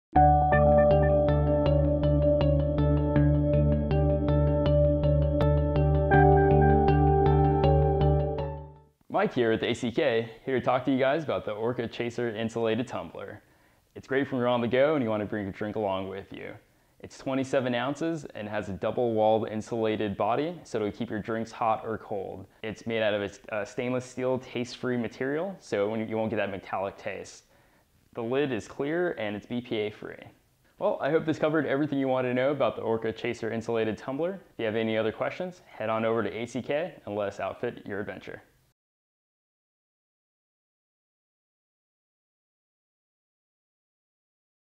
ACK Product Focus: Orca Chaser Tumbler
Whether you are going to work, the ball field, a tailgate, into the woods, down the dock, or to a party, the ORCA Chaser believes in getting you there with an ice-cold (or piping hot) beverage. The ORCA Chaser's stainless steel double walled vacuum sealed body ensures that your ice will stay frozen and your drink will stay strong, while its clear lid makes it easy to see when it's time for a refill.  today!